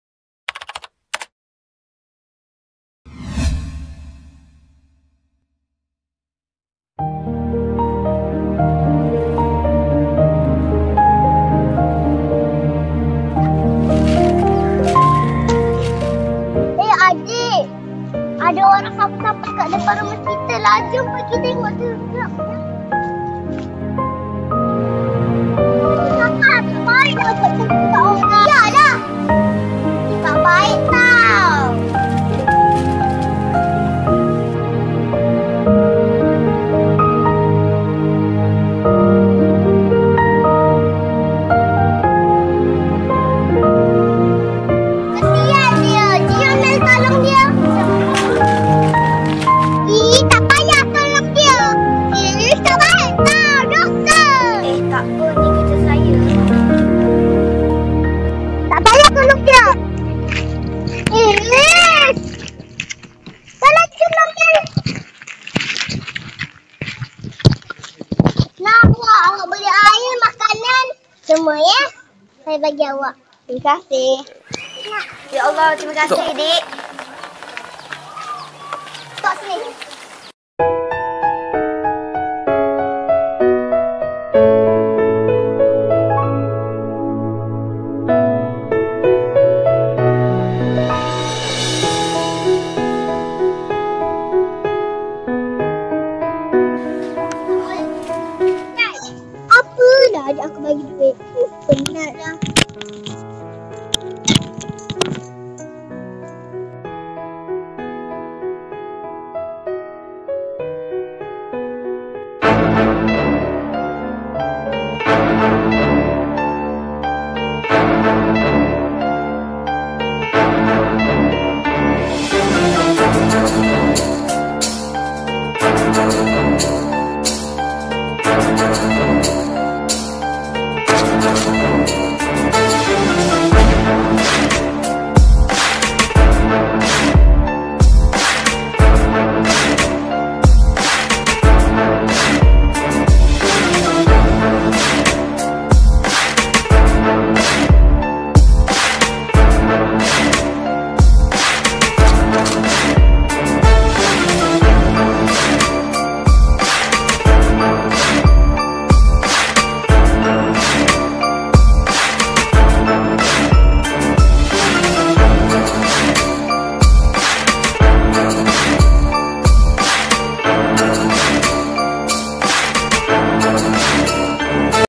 🔴ShortFilm spontan : Berbuat Jahat Akhirnya Memakan Diri | eps 2
Episode ke 2
Shortfilm yg Ditaja : Rodial M
Film pendek ini menceritakan seorang anak kecil miskin yg di bully oleh anak yg tajir,
N di saat itu masih ada juga adik yg kecil punya hati yg mulia berbuat baik. dalam masa yg sama si tajir merampas harta si miskin,dgn keseronakan itu juga si tajir itu kehilangan semua harta yg di miliki saat itu,,,

Sesungguhnya sesuatu akan berlaku lebih besar bila kita merampas hak milik orang lain..
Maka berbuat lah baik n perbnyakkanlah untuk bersedekah...

di bintangi :qaseh,qasrina,Qiesha n Emelly.

location di Kampung Nelayan.